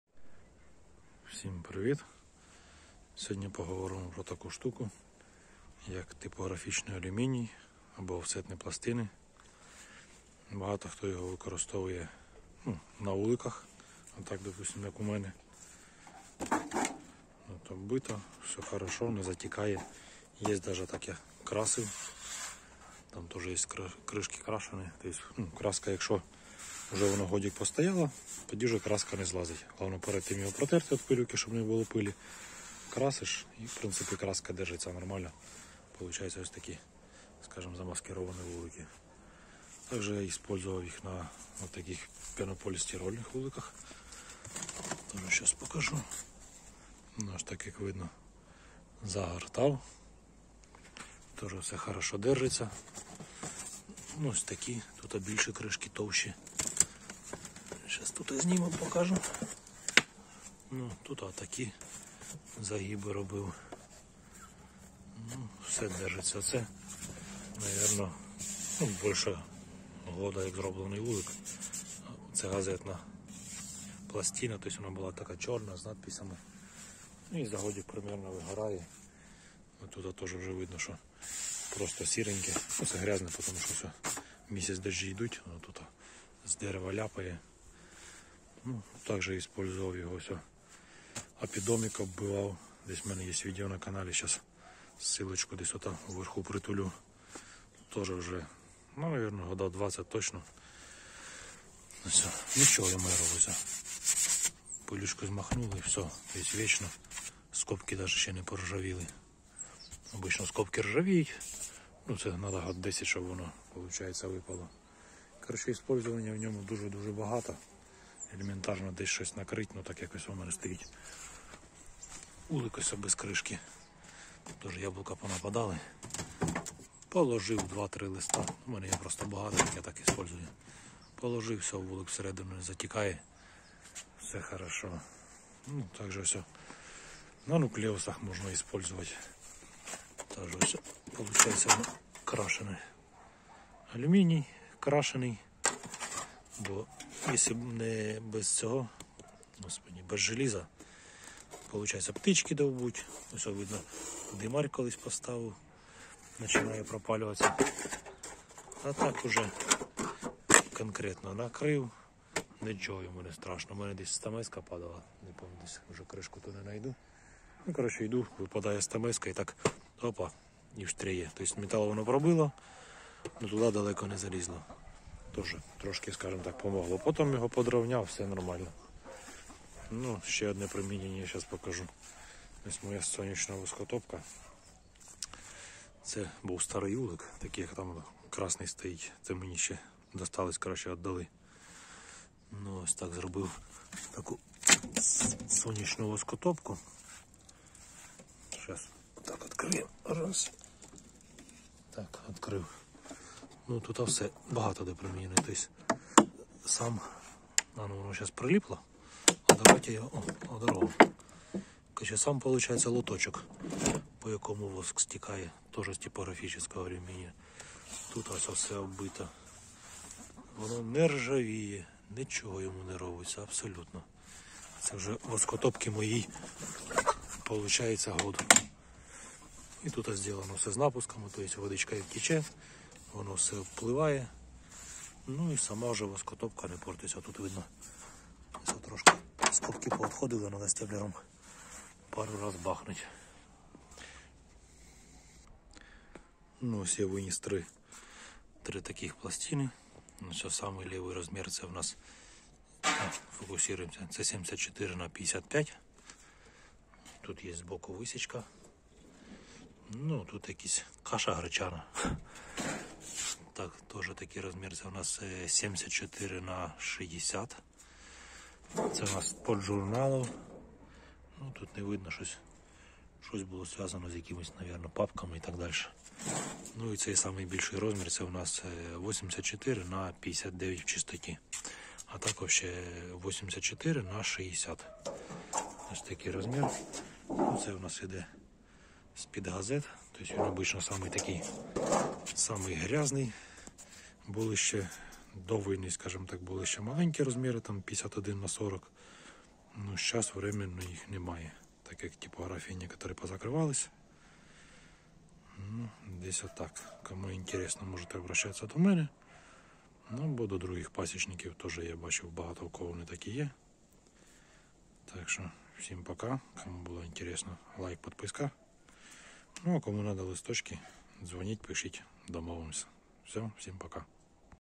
Чим покрити вулики. Типографічний алюміній для дахів вуликів і не тільки. Варіанти використання !!!
Бджолярі при заміні вуликів стикаються з проблемою чим покрити дахи. Багато хто використовує оцинковку, але її якість не дуже хороша і через пару років починає кородувати, ржавіти. Особливо, якщо вулики стоять в саду під фруктовими деревами. Також використовують рубероїд, лінолеум та багато іншого. Але всі ці матеріали недовговічні і не мають стійкості до корозії та механічних пошкоджень.
Вже багато років на своїй пасіці використовую дуже хороший матеріал: "Типографічний алюміній, офсетні пластини". Міцний, легкий, добре гнеться та обробляється, має високу стійкість до корозії, не вступає в реакцію з навколишнім середовищем. В себе на пасіці використовую його скрізь: на дахах вуликів, нуклеусів, 6-рамочних ППСах, також для оббиття апібудиночку, захисту дерев'яної сонячної воскотопки. Такому матеріалу можна знайти ще 1000 застосувань. Є декілька розмірів, треба уточнювати.
Даний матеріал можна замовити в мене по телефону або в Viber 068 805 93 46.